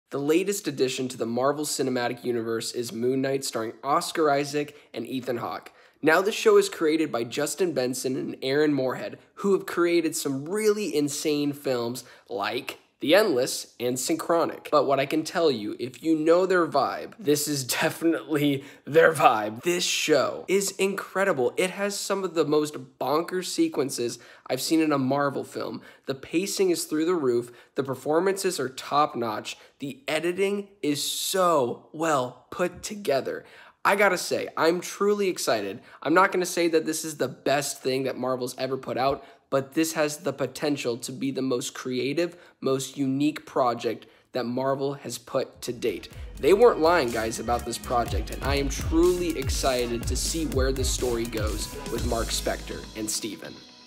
Moon Knight Episode 1 | Quick Review
Moon Knight starring Oscar Issac, Ethan Hawke, & F. Murray Abraham. Directed by Mohamed Diab, Justin Benson, & Aaron Moorhead.

This channel and I do not claim any rights over any of the graphics, images, audio, videos used in this video.